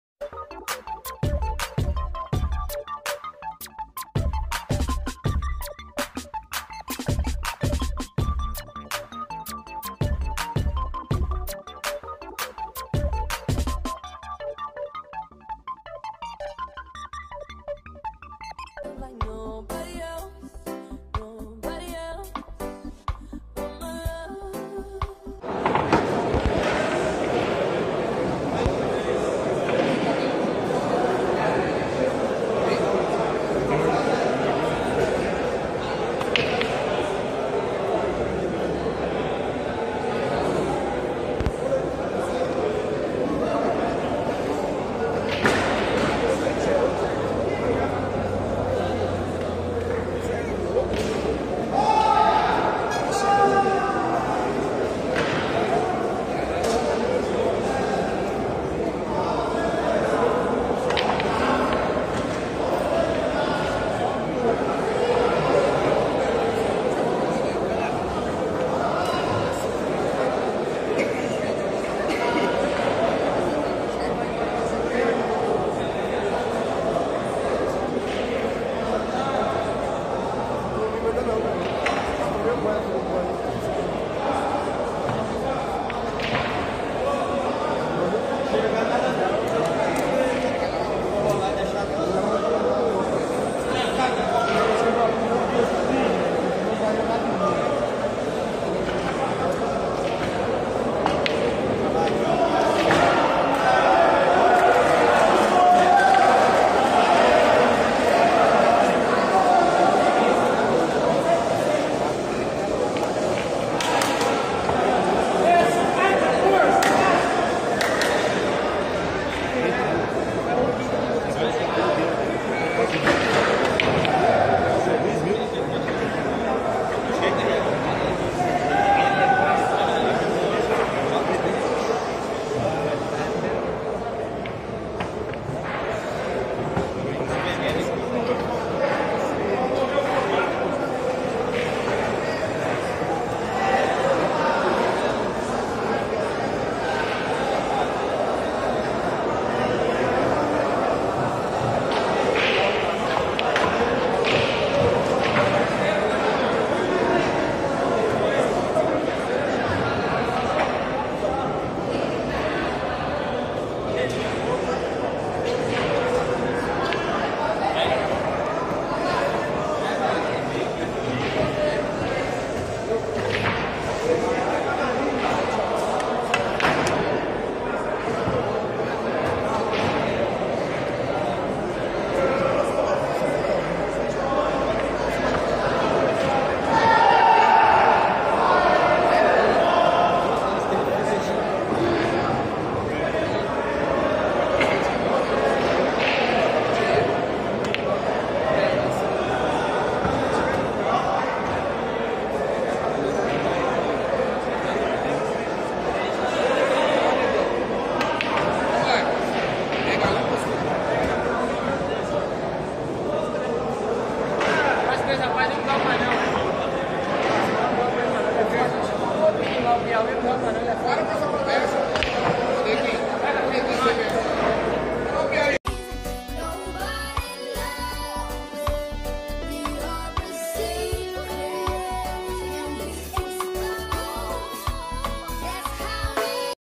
Maycon Teixeira dando um partido doido, e olha o que acontece!
Maycon Teixeira dar uma bola para matar 7 bolas no desafio quem faz 3 valendo 500,00